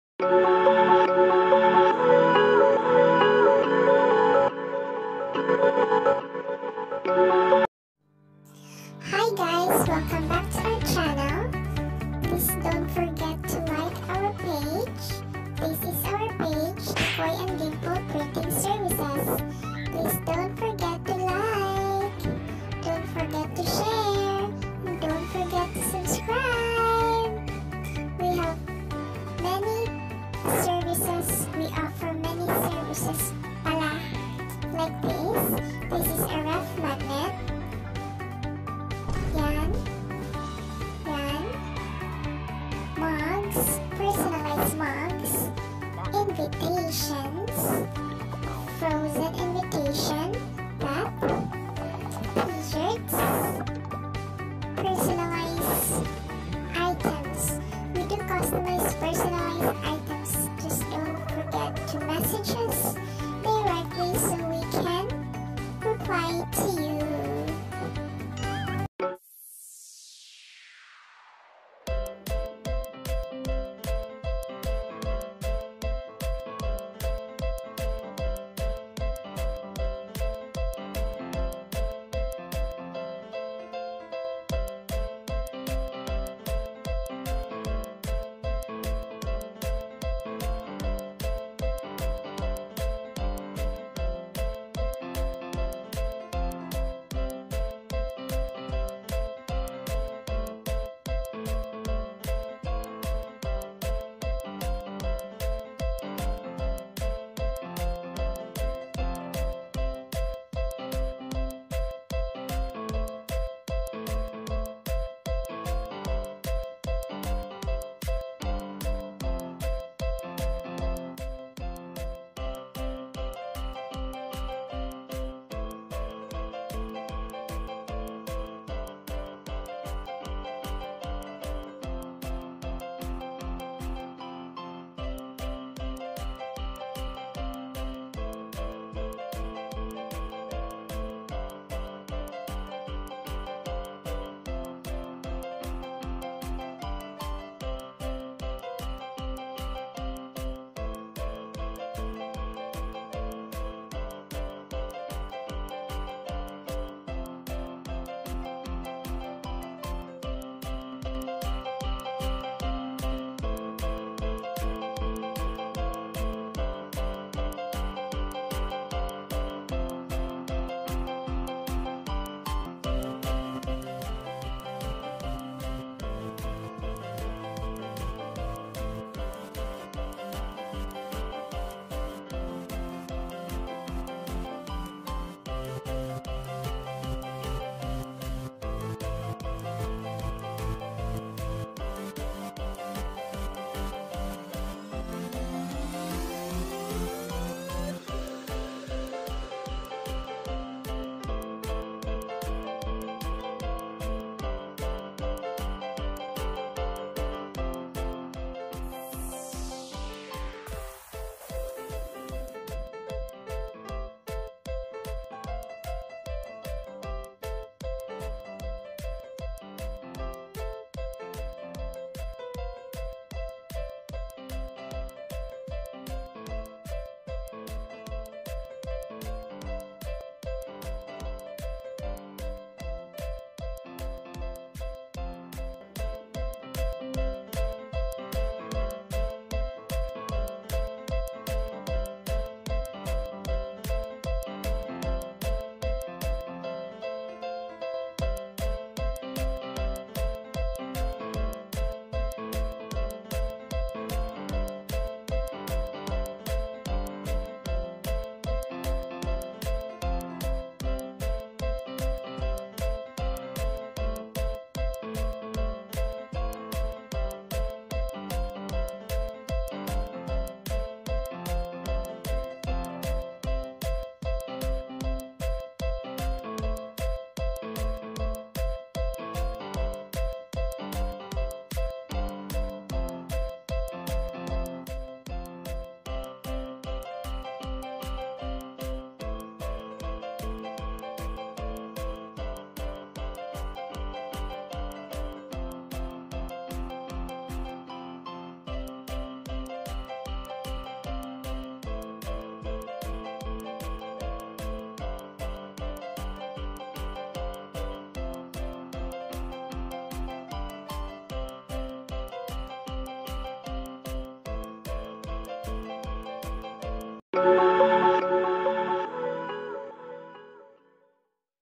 EASY PICSART OFFLINE TUTORIAL Android Phone | Dimples
Popart effect using Picsart. Easy way to make design of your pictures.